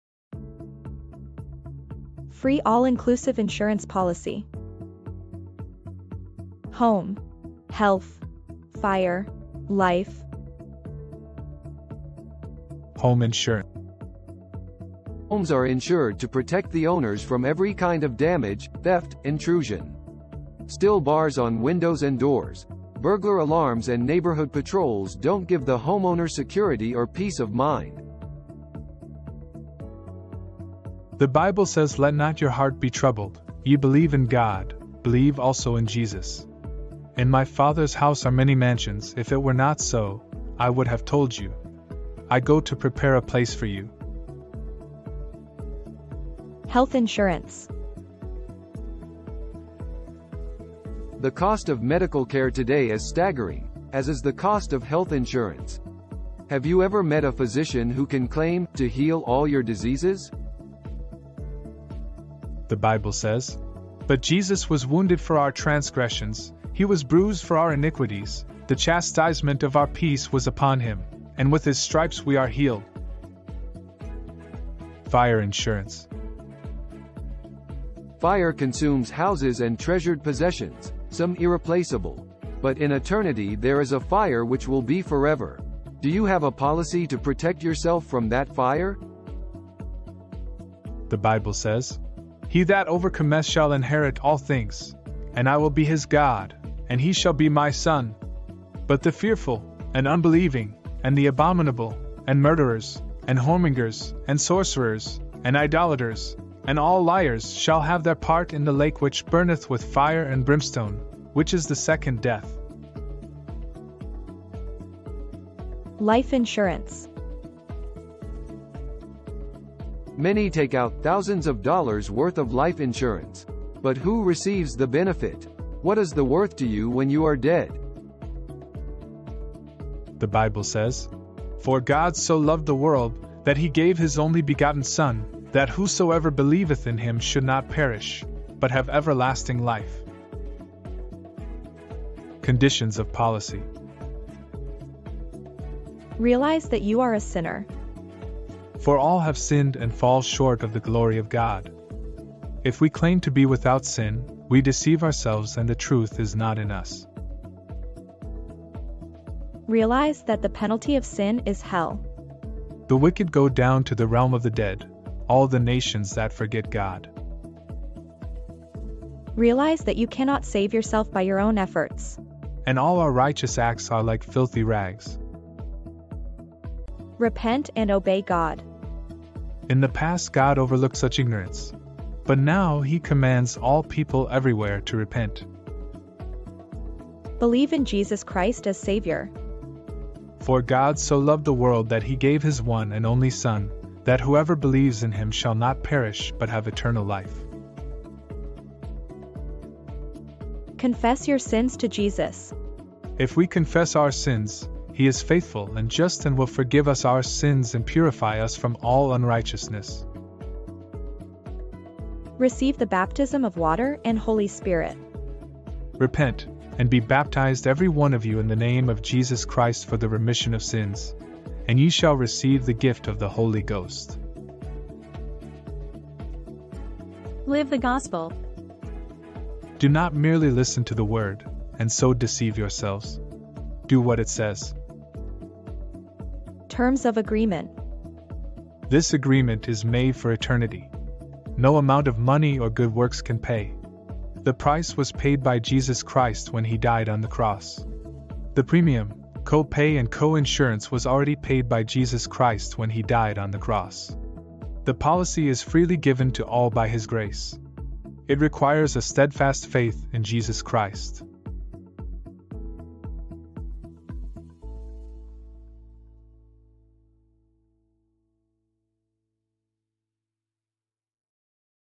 Free All inclusive Insurance Policy
Christian Video- Free All inclusive Insurance Policy,  Christian Gospel video tracts

Health

Home

Fire

Life

Good news Insurance
Health

The cost of medical care today is staggering, as is the cost of HEALTH insurance. Have you ever met a physician who can claim ‘to heal all your diseases?’
“But he was wounded for our transgressions, he was bruised for our iniquities: the chastisement of our peace was upon him; and with his stripes we are healed.” Isaiah 53:5

Home

Homes are insured to protect the owners from every kind of damage, theft, intrusion. Still bars on windows and doors, burglar alarms and neighborhood patrols don’t give the homeowner security or peace of mind.
“Let not your heart be troubled: ye believe in God, believe also in me. In my Father’s house are many mansions: if it were not so, I would have told you. I go to prepare a place for you” John 14: 1, 2

Fire

Fire consumes houses and treasured possesions, some irreplaceable. But in eternity there is a fire which will be forever. Do you have a policy to protect yourself from that fire?
“He that overcometh shall inherit all things; and I will be his God, and he shall be my son.
 But the fearful, and unbelieving, and the abominable, and murderers, and whoremongers, and sorcerers, and idolaters, and all liars, shall have their part in the lake which burneth with fire and brimstone: which is the second death “ Rev 21 : 7,8

Life

Many take out thousands of dollars worth of life insurance. But who receives the benefit? What is the worth to you when you are dead?

Conditions of policy
1. Realize that you are a sinner
“For all have sinned and fall short of the glory of God,” Rom 3:23
“If we claim to be without sin, we deceive ourselves and the truth is  not in us.”   1 John 1 :8

2. Realize that the penalty of sin is hell
“The wicked go down to the realm of the dead, all the nations that forget God.”   Psalms 9 : 17

3. Realize that you cannot save yourself by your own efforts
“And all our righteous acts are like filthy rags;….”   Isaiah 64 : 6

4. Repent and obey God
“In the past God overlooked such ignorance, but now he commands all people everywhere to repent. “ Acts 17:30

5. Believe in Jesus Christ as Savior

6. Confess your sins to Jesus
“If we confess our sins, he is faithful and just and will forgive us our sins  and purify us from all unrighteousness.” I John 1: 9

7. Receive the baptism of water and Holy Spirit
“Repent, and be baptized every one of you in the name of Jesus Christ for the remission of sins, and ye shall receive the gift of the Holy Ghost.” Acts 2 : 38

8. Live the Gospel
“Do not merely listen to the word, and so deceive yourselves.  Do what it says.” James 1 :22

Terms of Agreement
This agreement is made for eternity. No amount of money or good works can pay. The price was paid by Jesus Christ when He died on the Cross. The premium, copay and coinsurance was already paid by Jesus Christ when He died on the Cross. The policy is freely given to all by His Grace (Eph 2 :8 ). It requires a steadfast faith in Jesus Christ (Eph 3 :17)